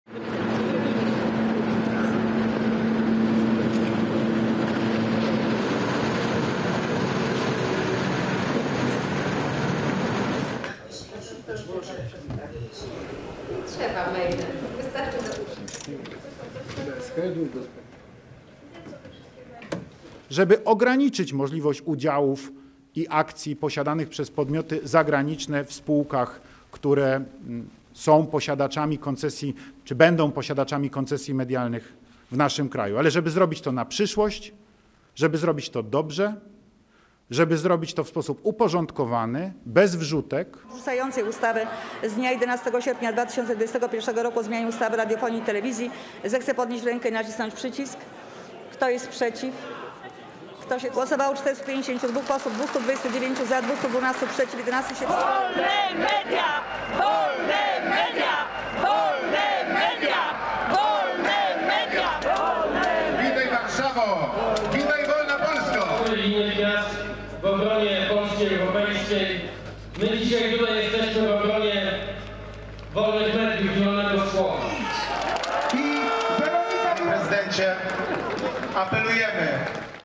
الرئيس البولندي يعطل قانونا مثيرا للجدل حول الإعلام
ويلزم القانون لو وقعه الرئيس، المجموعة الأمريكية "ديسكوفري" ببيع حصتها في "تي في ان" إحدى أكبر شبكات التلفزيون الخاصة في بولندا. و"تي في ان24" هي قناتها الإخبارية التي تبث على مدى 24 ساعة وتنتقد المحافظين الحاكمين.